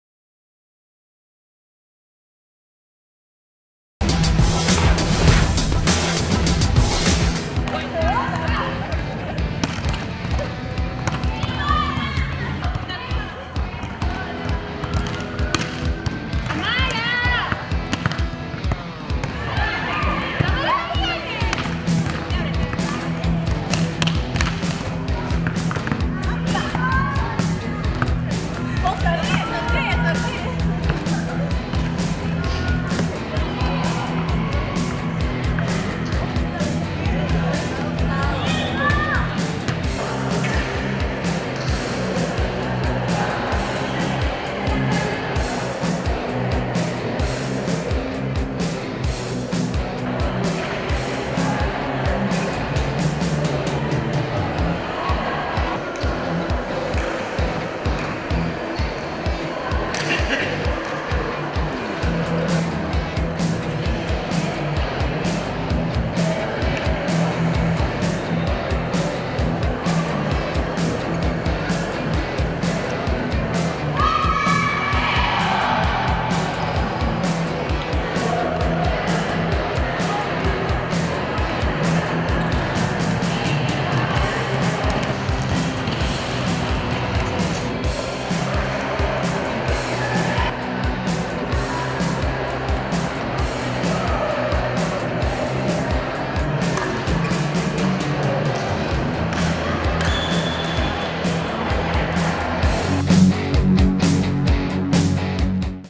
Así se vivió la previa del Vallejo vs Deportivo Alianza
Así se vivió la tercera fecha de la Liga Juvenil de Vóley entre el duelo de la Universidad César Vallejo vs. Deportivo Alianza.

UCV Deporte con su único estilo te trae los mejores momentos de la previa.

Radio UCV Deportes de Lunes a Viernes a las 11:30 p.m.